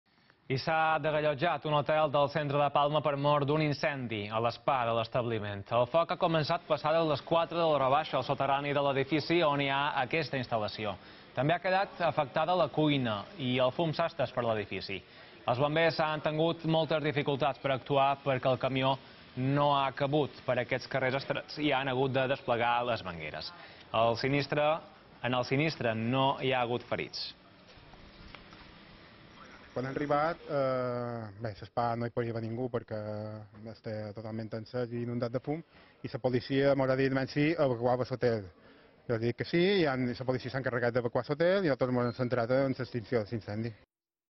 Incendi a l’spa d’un hotel del centre de Palma
S’ha desallotjat un hotel del centre de Palma per mor d’un incendi a l’spa de l’establiment. El foc ha començat passades les quatre de l’horabaixa al soterrani de l’edifici, on hi ha aquesta instal·lació. També ha quedat afectada la cuina, i el fum s’ha estès per l’edifici.

Els bombers han tengut moltes dificultats per actuar, perquè el camió no ha cabut per aquests carrers estrets i han hagut de desplegar les mangueres. En el sinistre no hi ha hagut ferits.